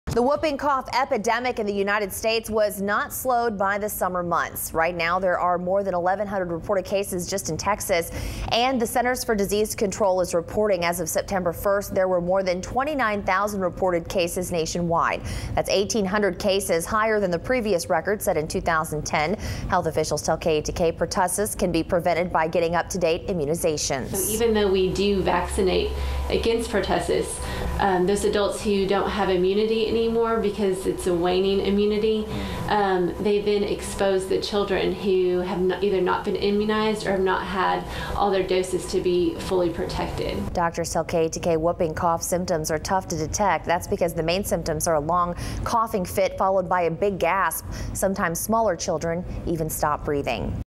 Whooping Cough Cases Increasing
(KETK) September 6, 2012

More than 1,100 cases have been reported in the state of Texas. As of September 1, the CDC is reporting more than 29,000 cases nationwide. That's 1,800 cases higher than the previous record set in 2010. Trinity Clinic Pediatrician Holly Blanco explains Pertussis (Whooping Cough) can be prevented by getting up to date immunizations.